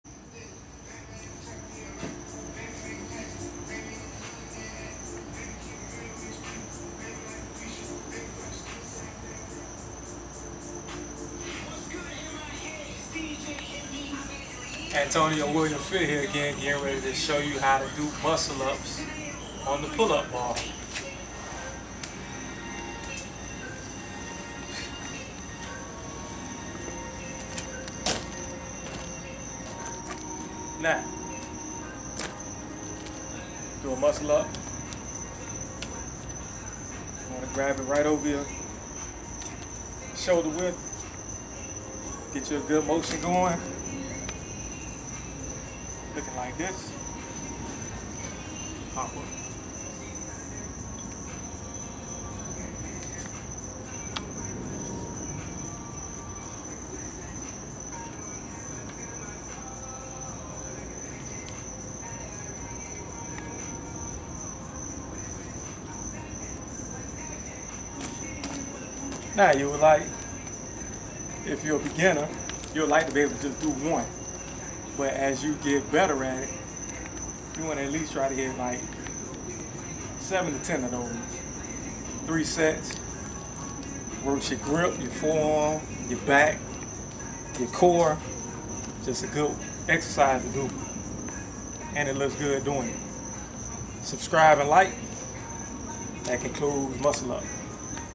Muscle Ups on a Pull up Bar
Try and do 7-10 reps, 4 sets. It gives you a total upper body workout.
30 reps, 4 sets on left, right, and center abs